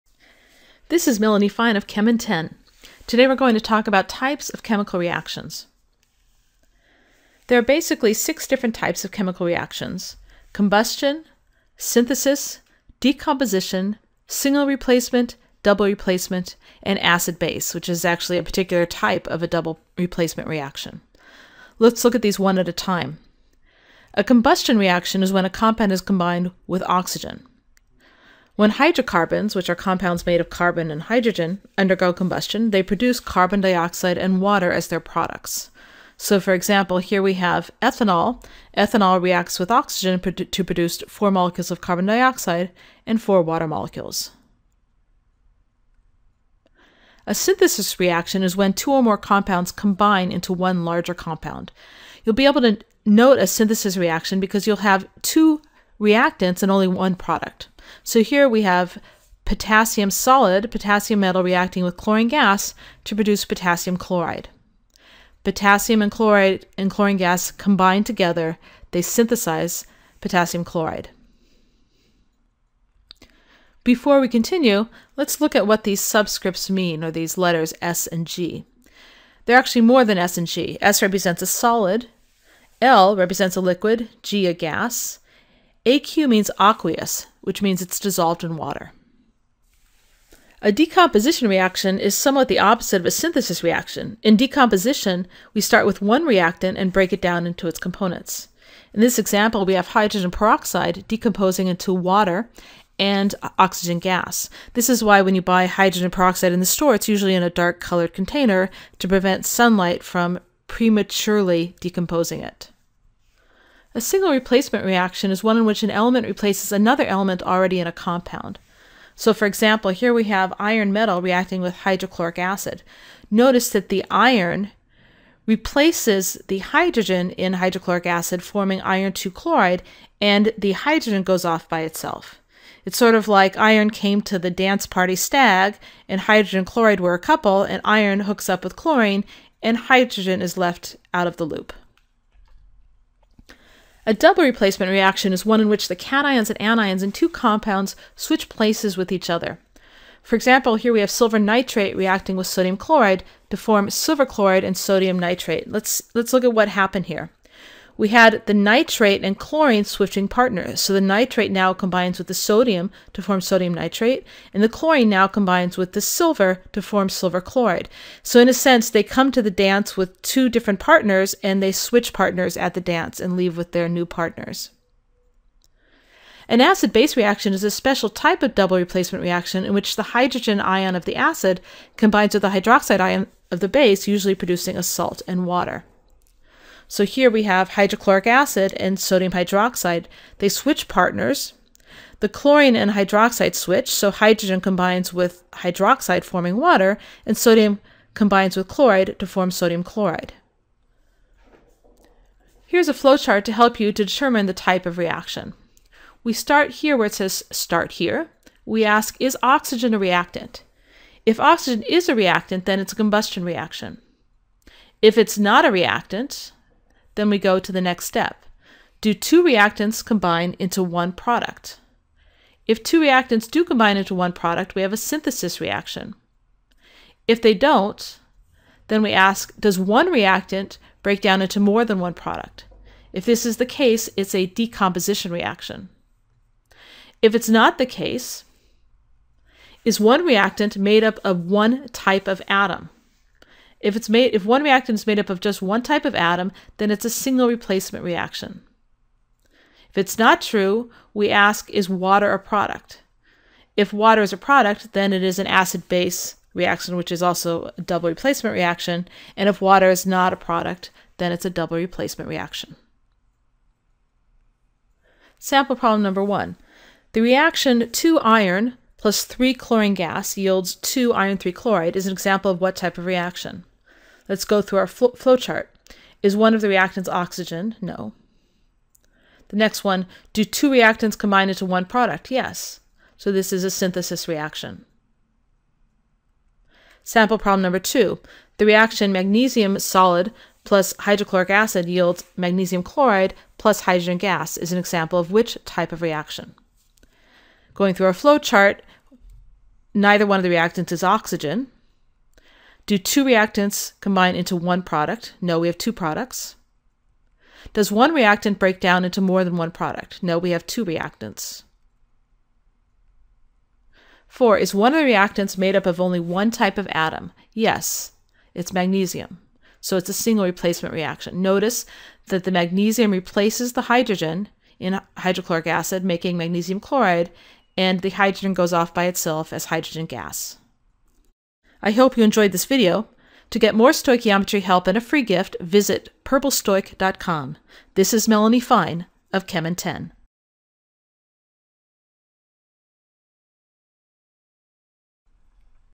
Types of Chemical Reactions: How to identify chemical reactions
There are 6 general types of chemical reactions which I describe in this video -- combustion, decomposition, synthesis, single replacement, double replacement and acid/base.  Using the flow chart in this video, I show you how to identify which type of reaction you're looking at.